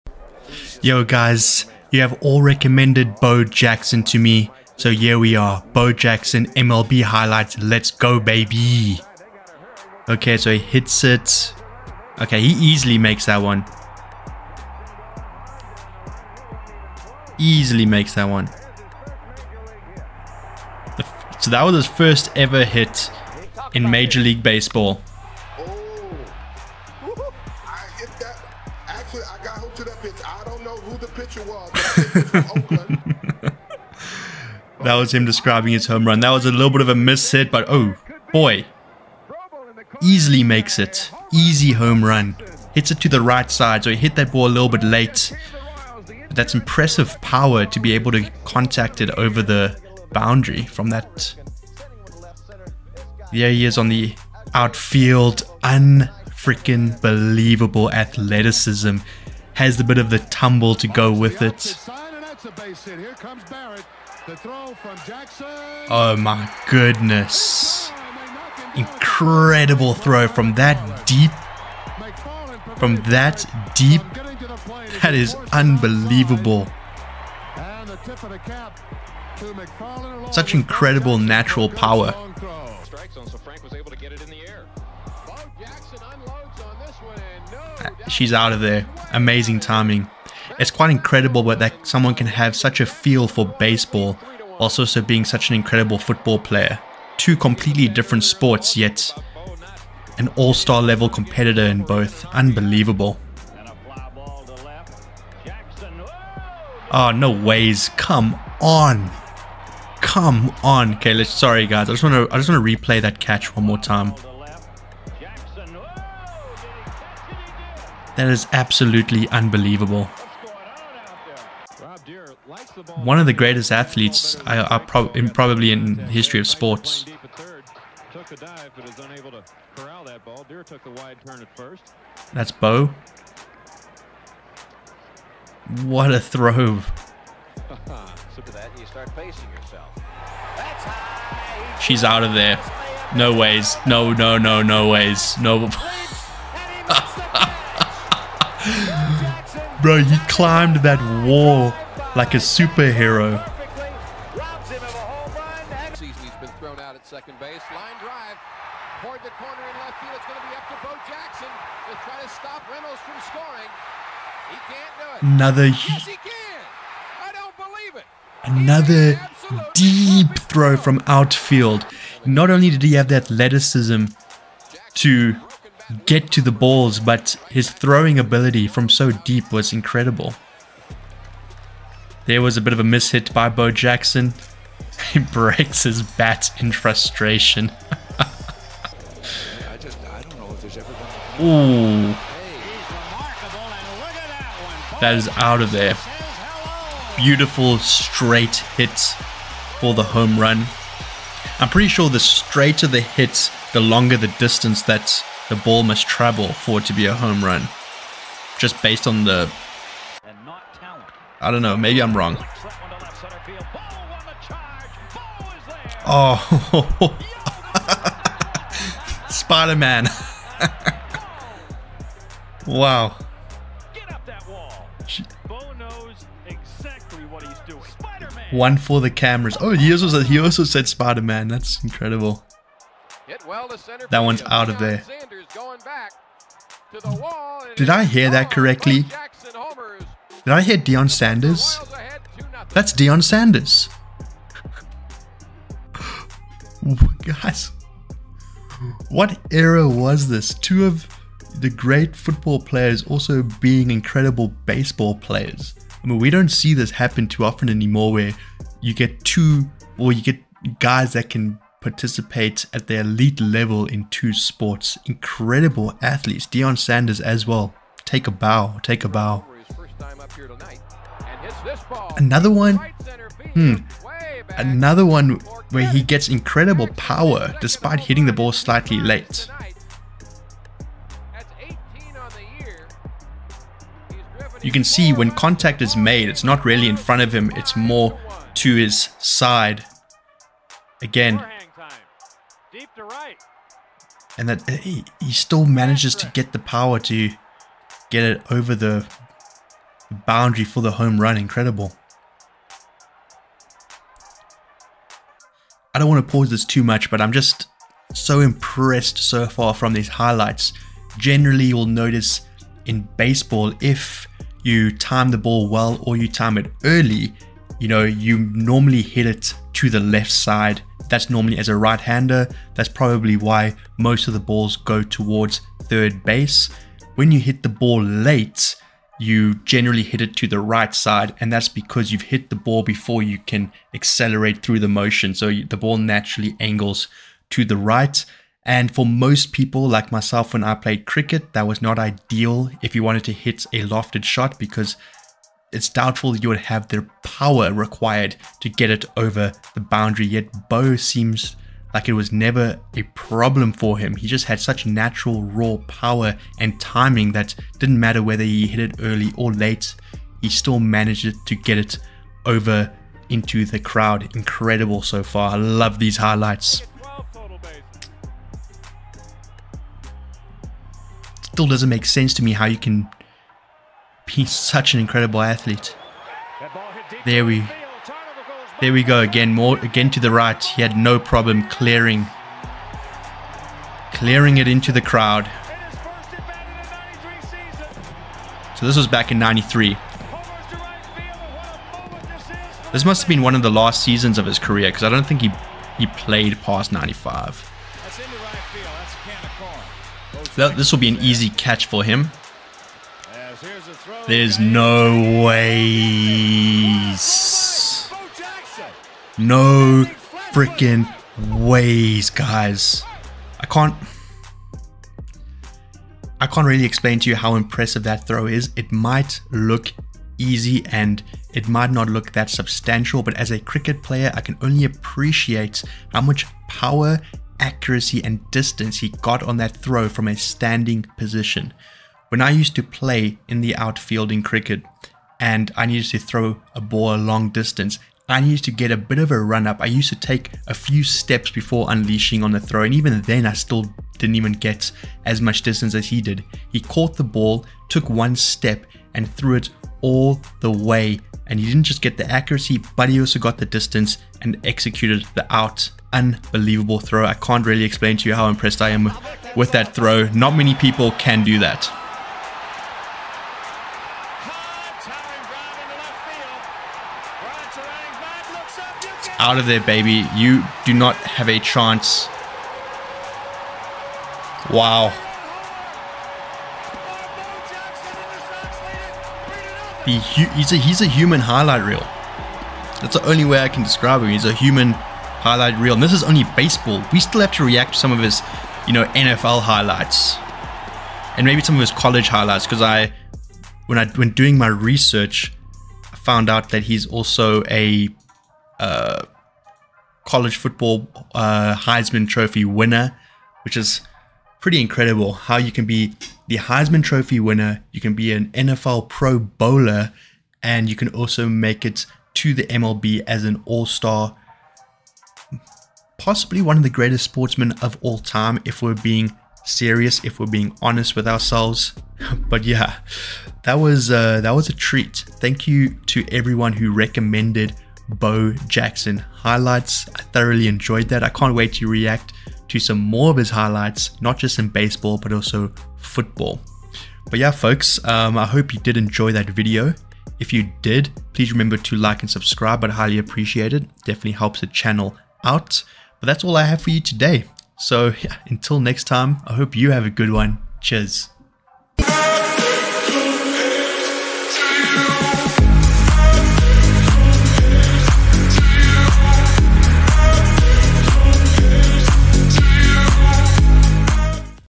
First Time Watching Bo Jackson !!! | MLB/Baseball Highlights | I am in SHOCK!!!
Reaction to Bo Jackson's baseball highlights. Bo Knows!!! What an unbelievable highlight reel.